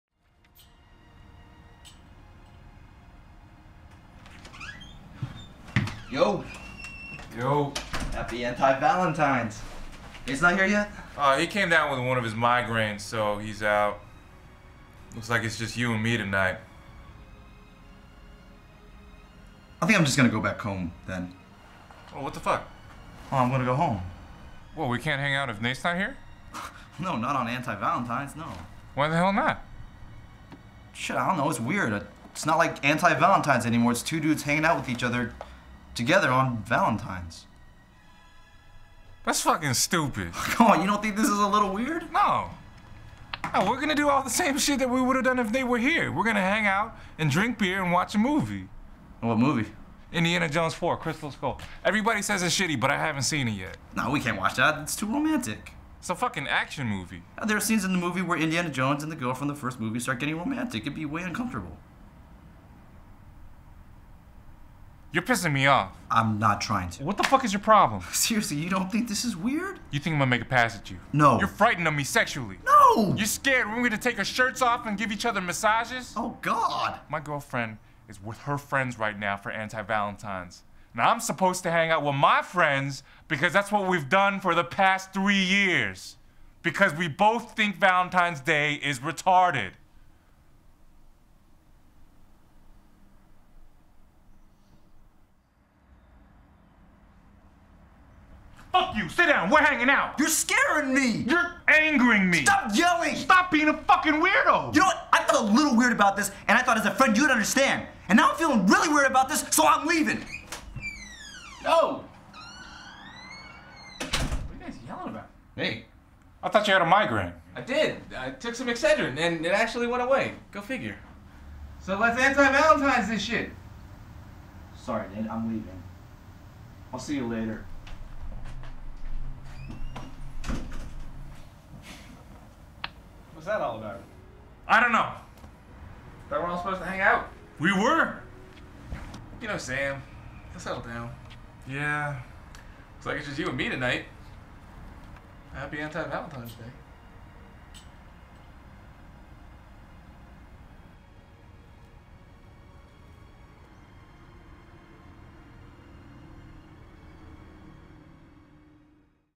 Anti-Valentines
Think Valentine's day can be complicated? Sometimes Anti-Valentine's isn't much easier.

Produced by Neil Sehgal
Written and Directed by David J. Lee
Cinematography by David S. Lee
Production Design by Tim Chiou
Titles and Credits by Jarrett Quon
Television Footage courtesy of David S. Lee
Special Thanks to Stephanie Jo Wu and Jarrett Quon
More Special Thanks to Shane Patterson and Mariana Flores

CAST:
Terry - Tim Chiou
Sam - Leonard Wu
Nate - Neil Sehgal